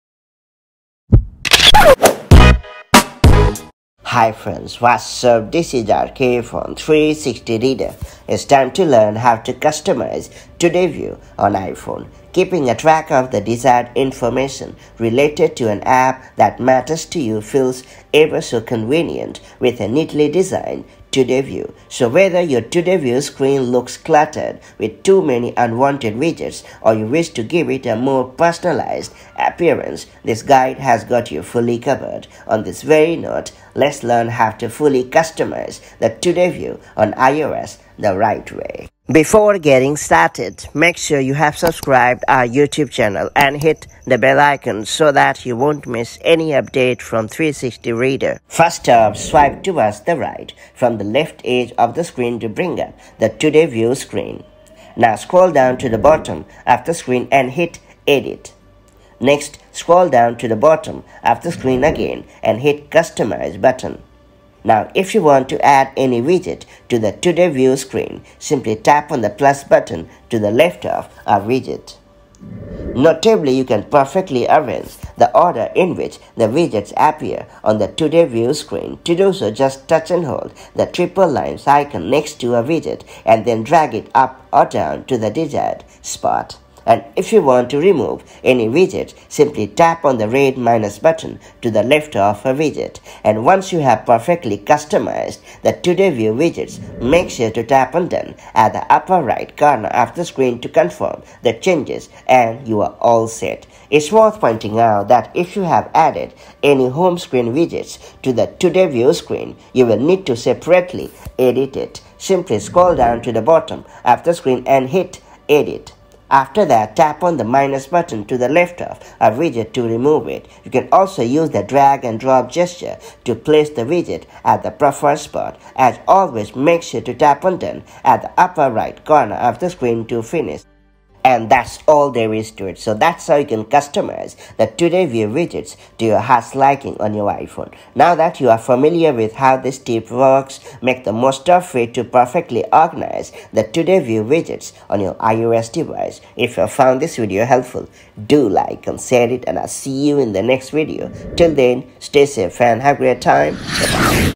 How to Customize Today View Widgets on iPhone in iOS 16 🔥🤩
Here is how you can perfectly customize Today View widgets in iOS 16 on your iPhone. Let's get started!